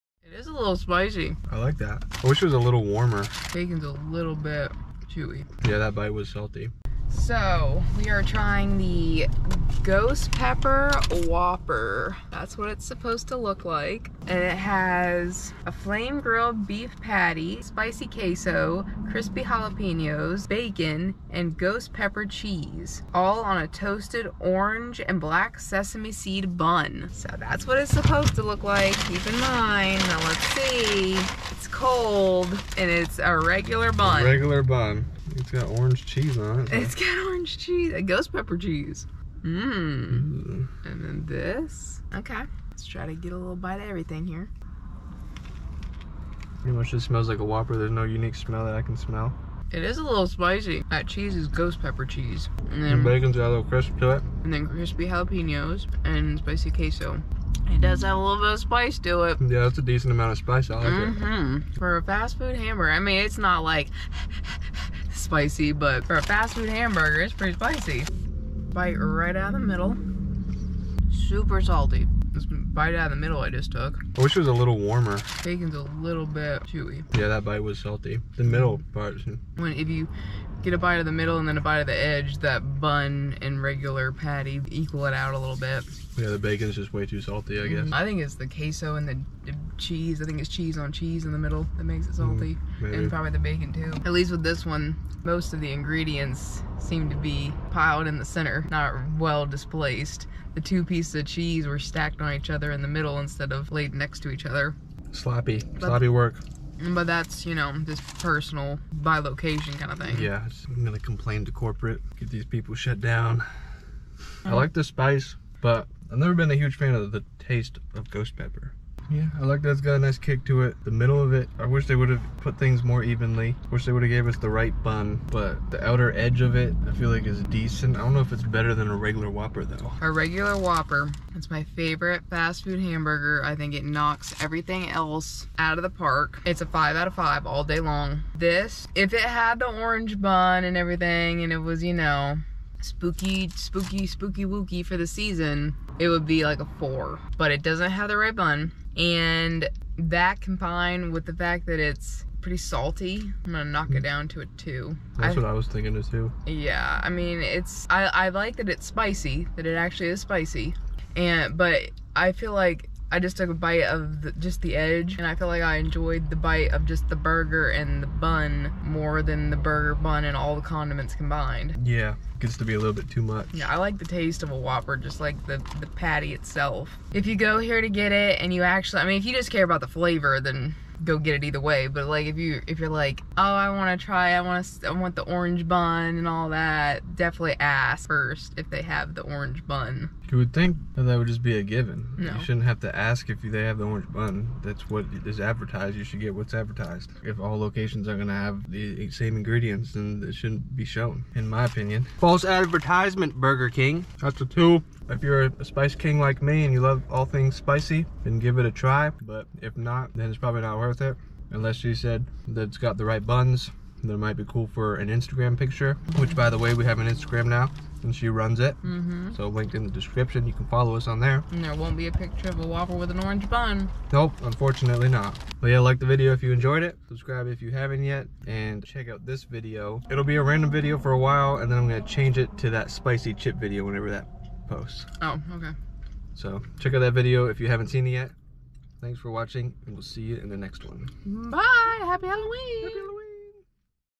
NEW Burger King Ghost Pepper Whopper Review!
We ate the NEW Burger King Ghost Pepper Whopper and gave our honest review! Some things are nice about it, but some things could definitely be improved. But overall it's a nice addition to this holiday snack season.